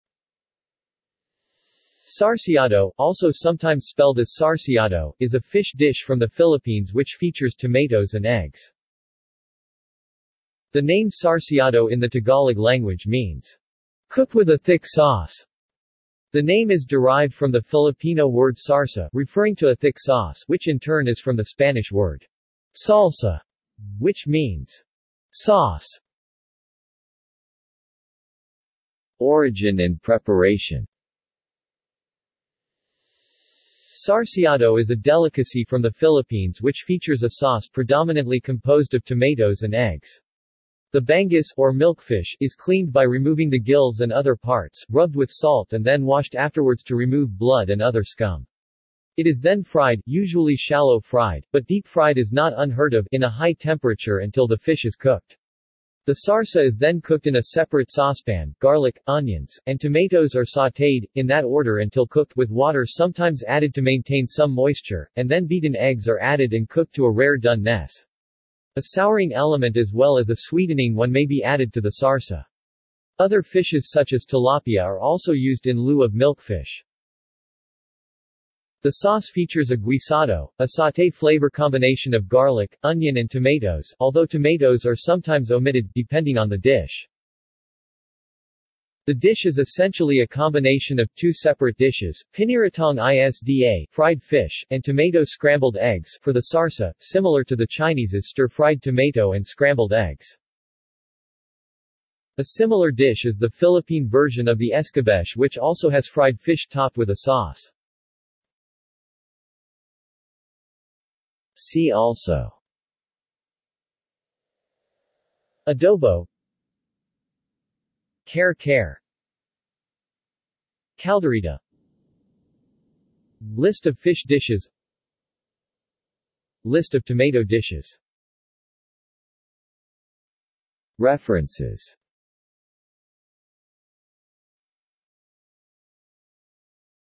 Sarsiado | Wikipedia audio article
This is an audio version of the Wikipedia Article:
Sarsiado

SUMMARY
Sarsiado (also sometimes spelled as sarciado) is a fish dish from the Philippines which features tomatoes and eggs.
The name sarsiado in the Tagalog language means "cooked with a thick sauce". The name is derived from the Filipino word sarsa (referring to a thick sauce) which in turn is from the Spanish word "salsa", which means "sauce".